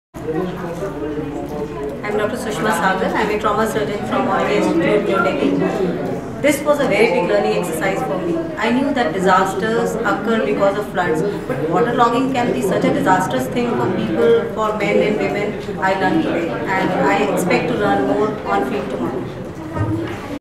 Dr Sushma Sagar, Additional Professor, Deptt of Surgical Disciplines, AIIMS
Dr Sushma Sagar, MS  FACS (Plast & Reconst), Additional Professor, Deptt of Surgical Disciplines, JPN Apex Trauma Center, AIIMS

UN SolEx Gender CoP Learning Event in Puri, Odisha, 7 - 10 August 2013